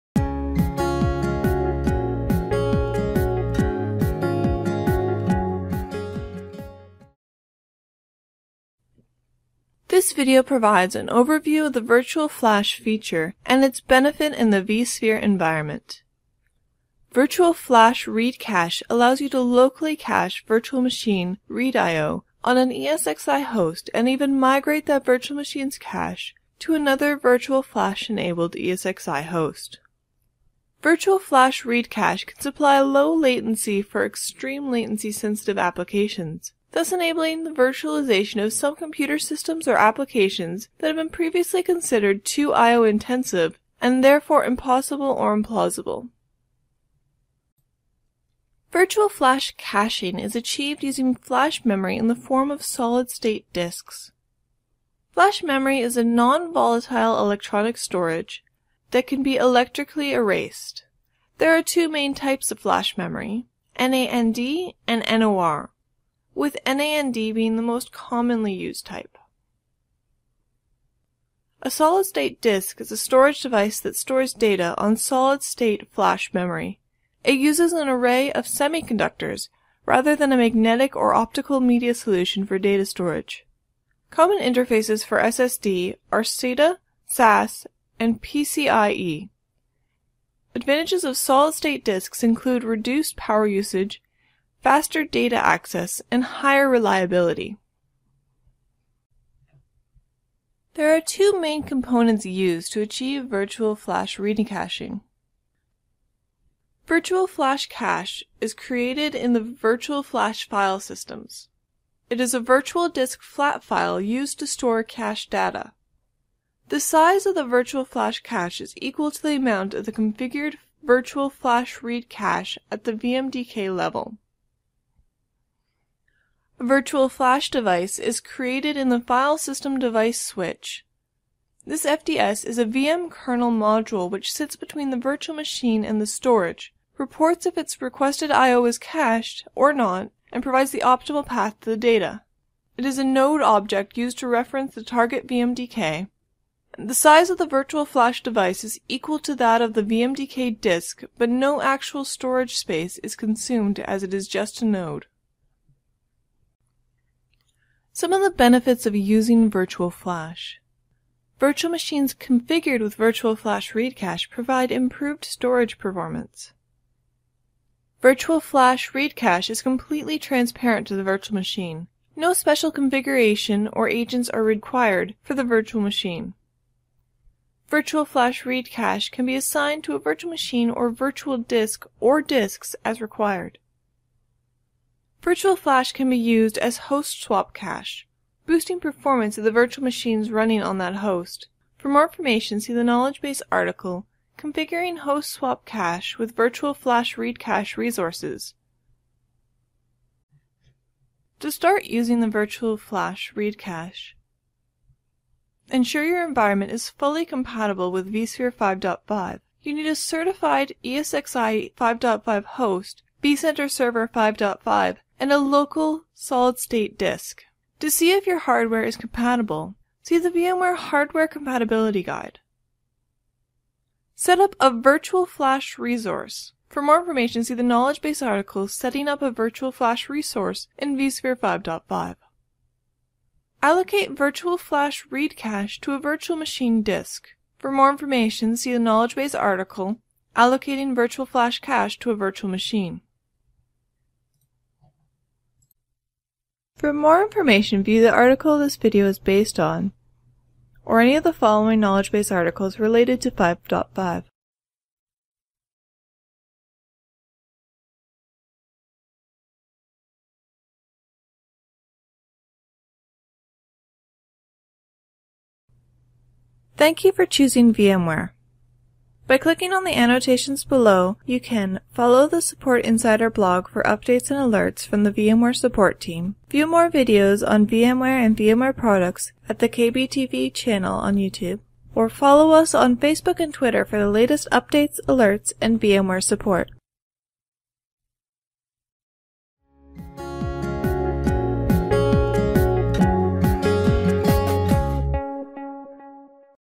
Virtual Flash feature in vSphere 5 5 KB 2058983
- This video discusses and provides information relating to the Virtual Flash feature in vSphere 5.5.

You will be presented with an overview of Virtual Flash and its benefit in the vSphere environment. Virtual Flash Read Cache allows you to locally cache virtual machine read I/O on an ESXi host and even migrate that virtual machines cache to another Virtual Flash enabled ESXi host.

Virtual Flash allows you to accelerate virtual machine performance through the use of local SSD disks, which serve flash memory cache to chosen virtual machines running on the ESXi host. Virtual Flash Read Cache can supply low latency for extreme latency sensitive applications, thus enabling the virtualization of some computer systems/applications previously considered too I/O intensive and, therefore, impossible/implausible.